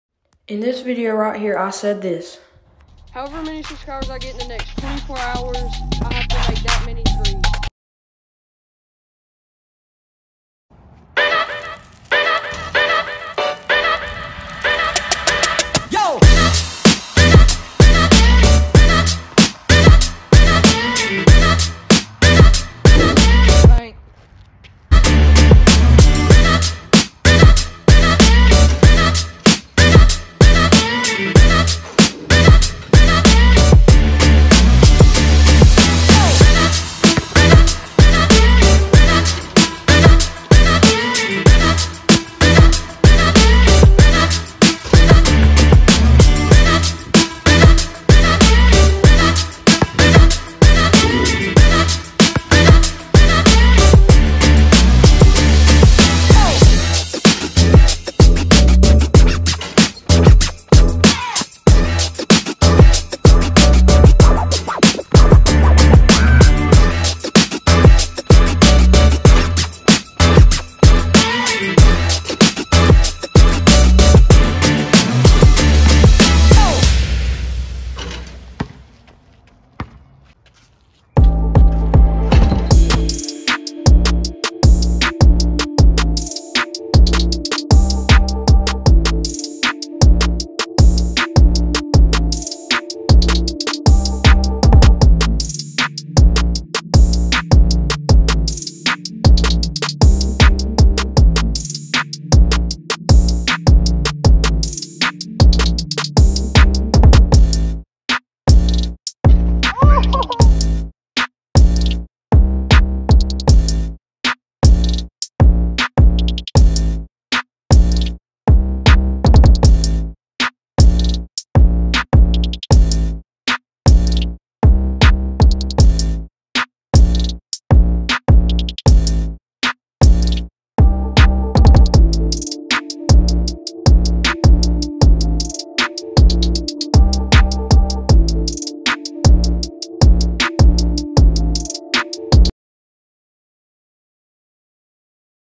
I made 36 threes,As I promised!
I made a challenge that I had to make as many subscribers I got in 24 hours.